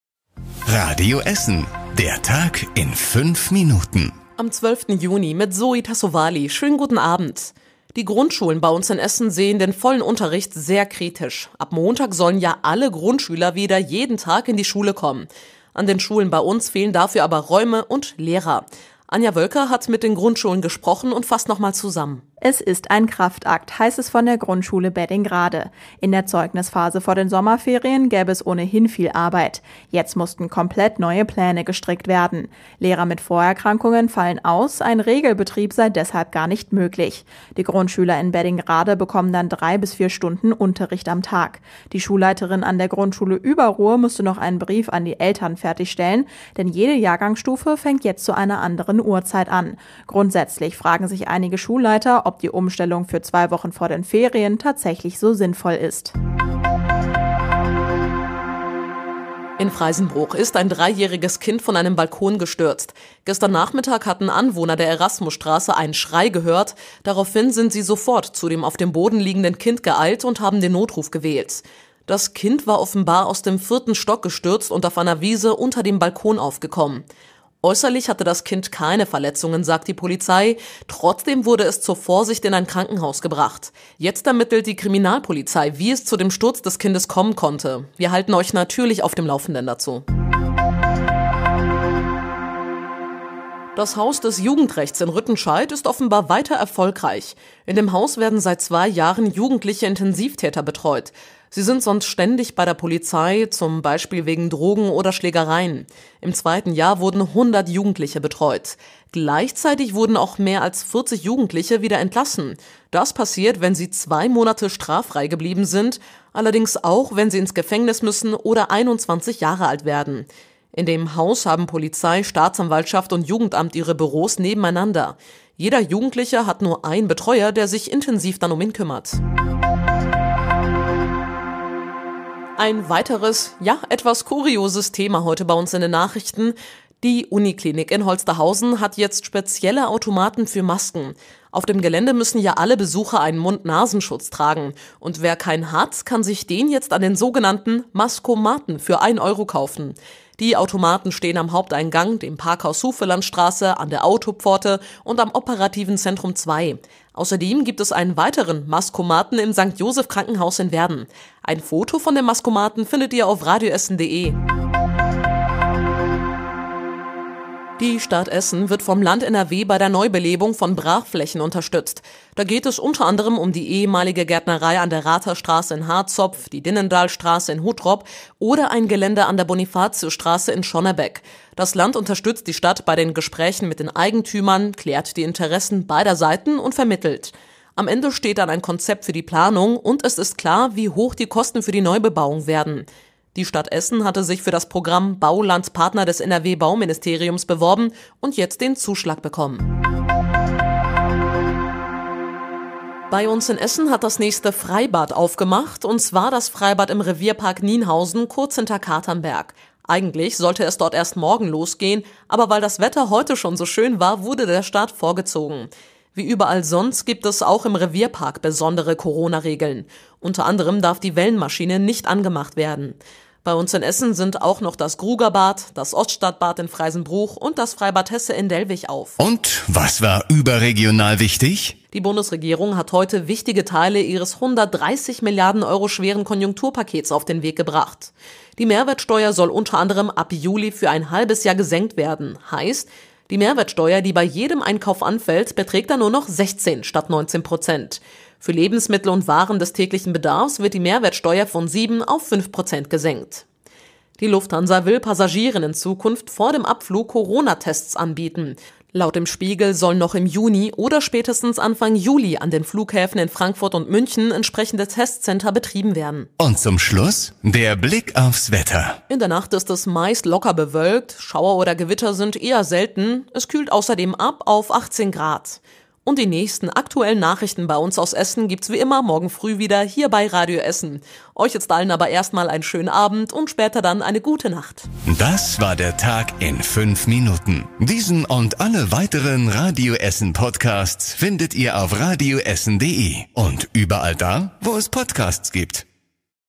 #142 Der 12. Juni in 5 Minuten - Der Tag in 5 Minuten
Die Grundschulen in Essen bereiten sich auf Montag vor. Dann sollen wieder alle Grundschüler in die Schule dürfen. Es gibt aber viel Kritik. In Essen ist ein dreijähriges Kind vom Balkon gestürzt. Die Bundesregierung hat wichtige Teile des Konjunkturpakets beschlossen.